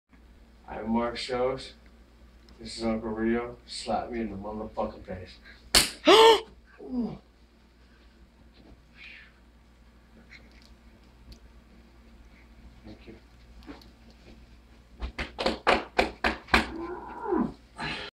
Mark Shows my best friend
He wanted me to slap him in the face